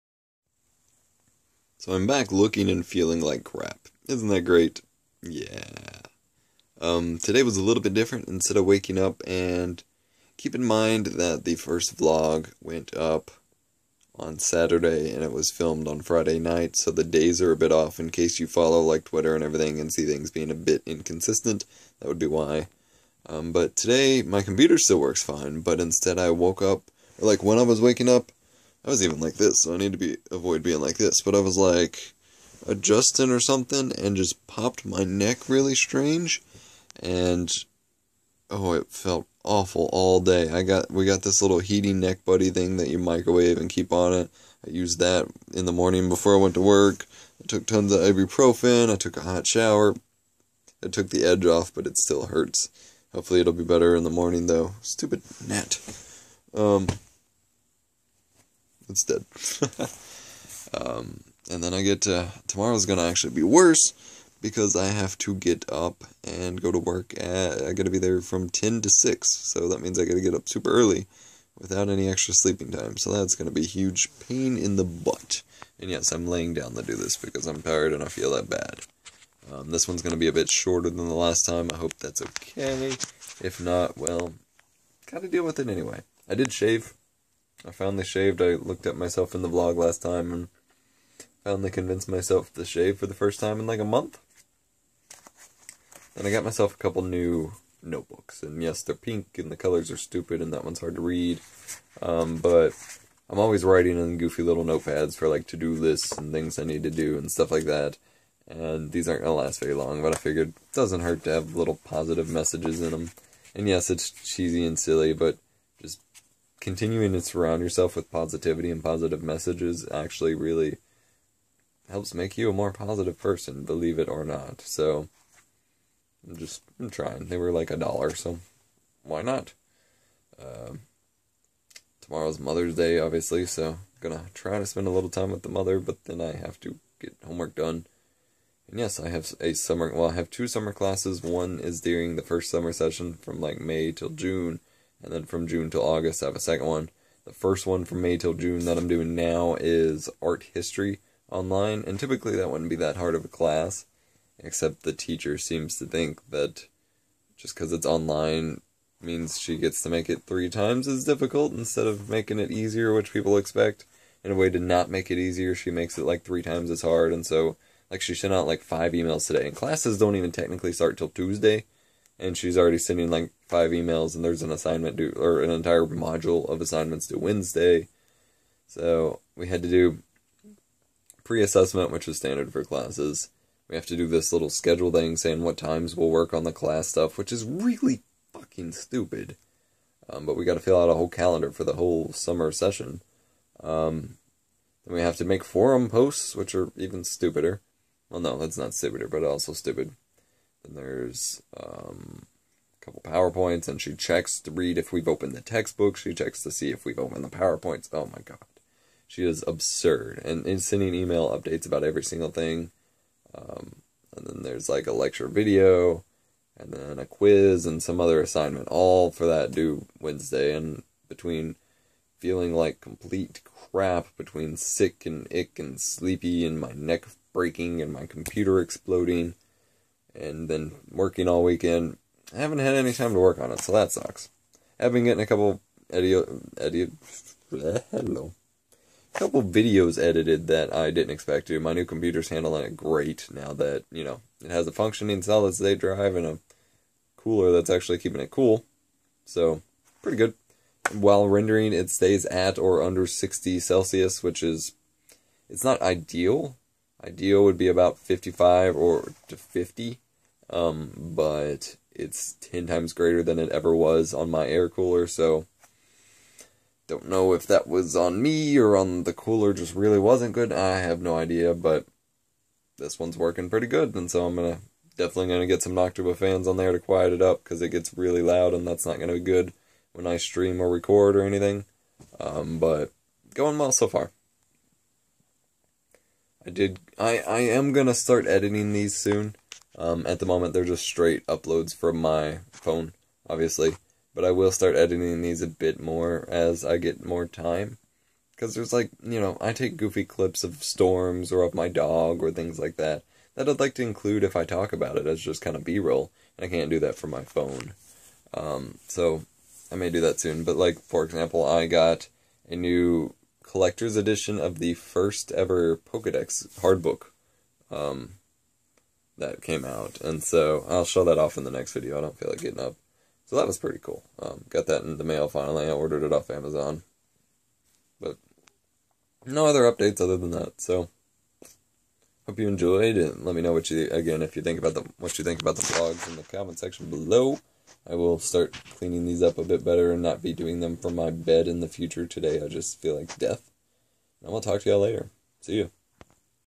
Feels Like Death
This video was funded by the best!

It's a vlog. Yes I look like shit. Okay.
As of updating the description my work weekend is over. Hopefully I can rest and recover.